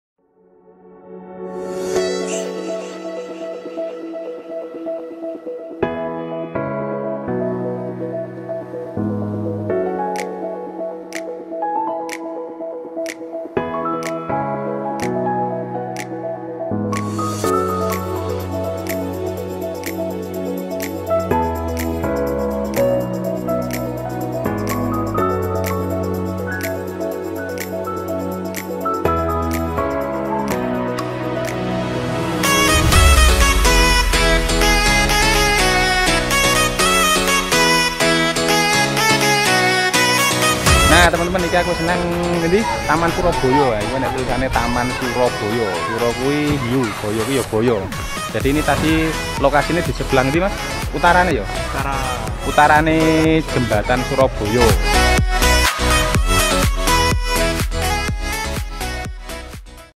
Jembatan dan Taman Suroboyo - Cuplikan DRONE VIEW
Video singkat Drone view Jembatan Suroboyo dan Taman Suroboyo.
Nantikan video lengkapnya di episode selanjutnya...
Cekibrooottttt.